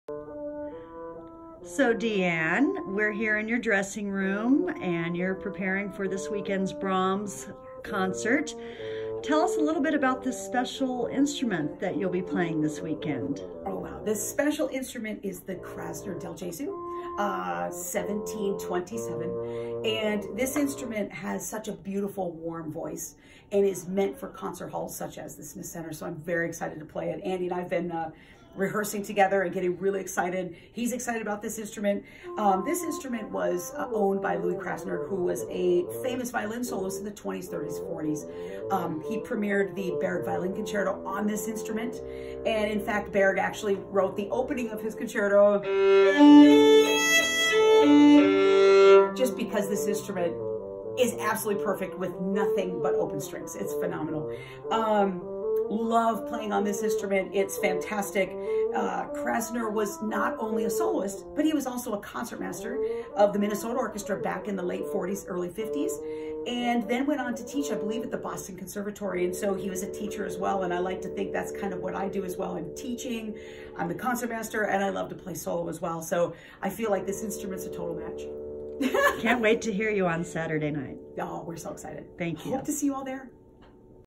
Concertmaster De Ann Letourneau shares the history behind the Guiseppe Guarneri ‘del Gesù’ violin
Concertmaster De Ann Letourneau shares the history behind the rare violin made by celebrated maker Guiseppe Guarneri ‘del Gesù’ that she will be playing at Saturday's An Evening of Brahms performance.